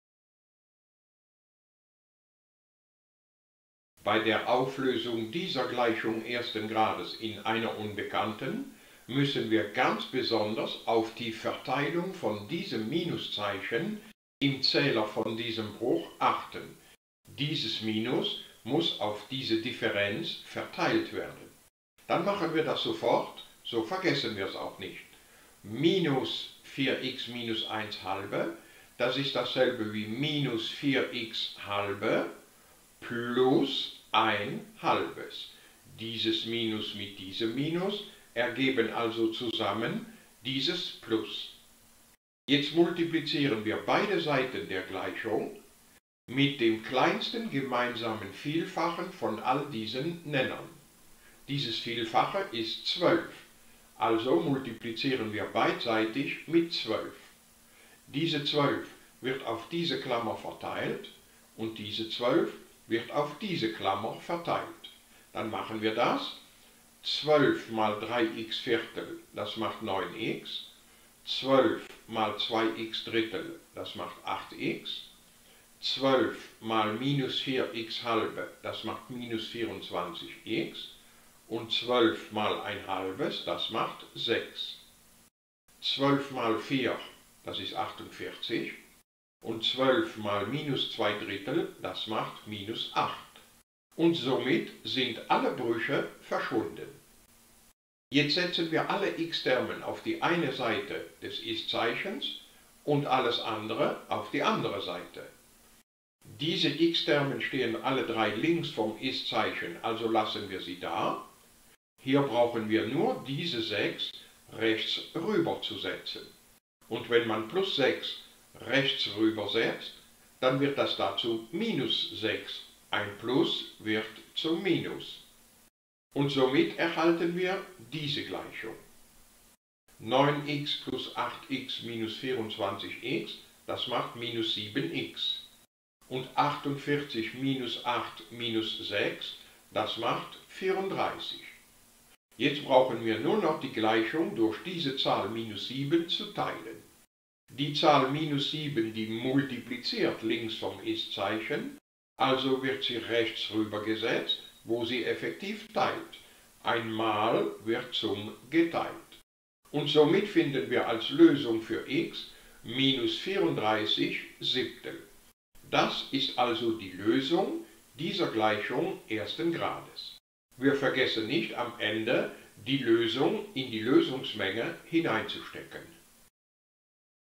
Gleichungen ersten Grades 008
Wir lösen hier eine Gleichung ersten Grades mit einer Unbekannten.